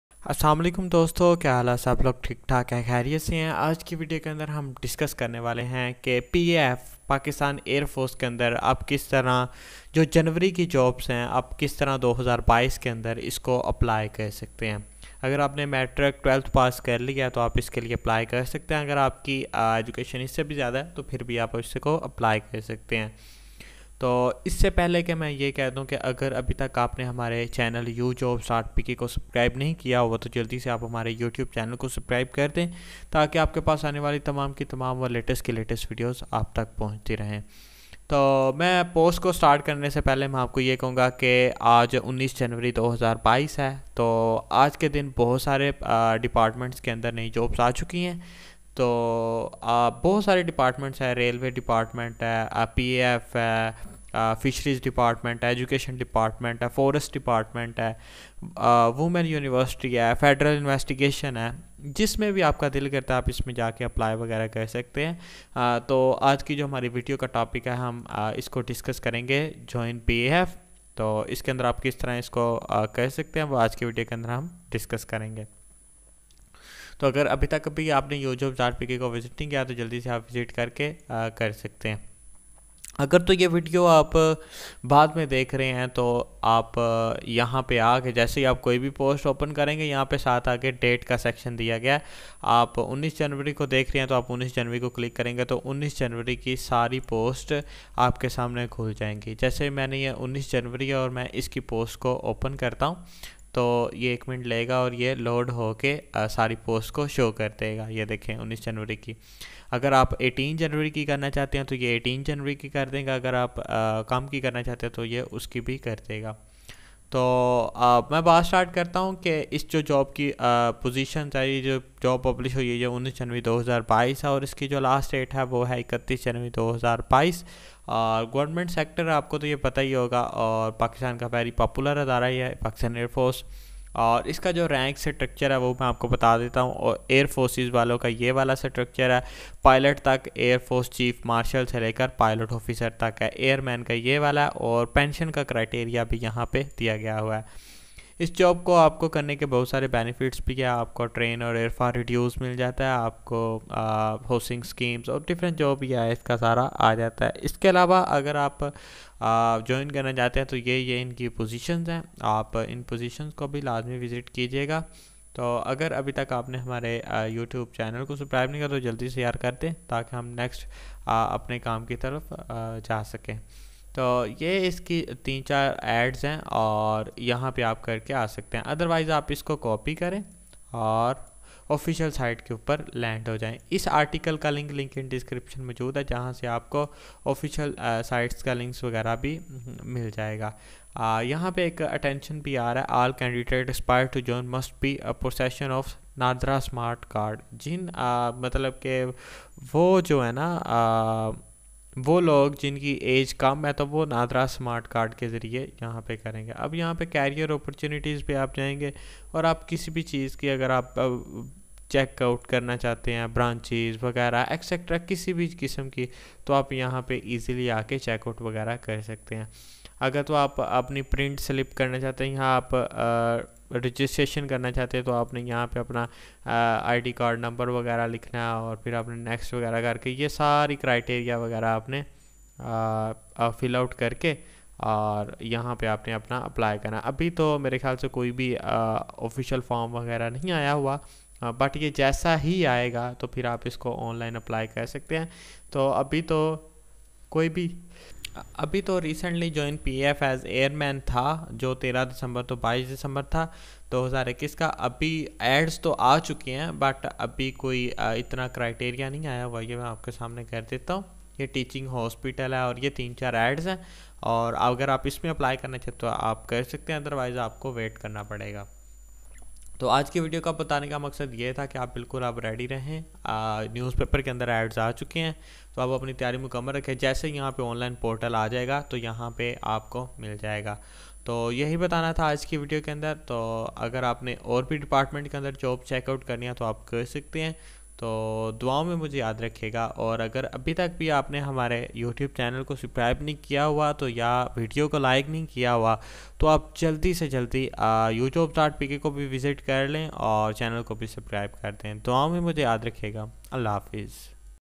Join PAF jobs 2022 – Join Pakistan Air Force After 10th or 12th Pass
پی اے ایف کی ملازمتیں 2022 میں شامل ہوں - 10ویں یا 12ویں پاس کے بعد پاکستان ایئر فورس میں شمولیت اختیار کریں

Pakistan Air Force PAF Offers Jobs Positions in Pakistan. Fresh and Experienced individuals are encouraged to apply. The latest Job is published on 19 Jan 2022 in the newspaper. The last date to apply for this job is  31 Jan 2022. More Details about the Jobs in the PAF Jobs 2022 are mentioned below in the advertisement.

پاکستان ایئر فورس پی اے ایف پاکستان میں نوکریوں کی پیشکش کرتی ہے۔ تازہ اور تجربہ کار افراد کو درخواست دینے کی ترغیب دی جاتی ہے۔ تازہ ترین ملازمت 19 جنوری 2022 کو اخبار میں شائع ہوئی ہے۔ اس نوکری کے لیے درخواست دینے کی آخری تاریخ 31 جنوری 2022 ہے۔ پی اے ایف جابز 2022 میں ملازمتوں کے بارے میں مزید تفصیلات نیچے اشتہار میں دی گئی ہیں۔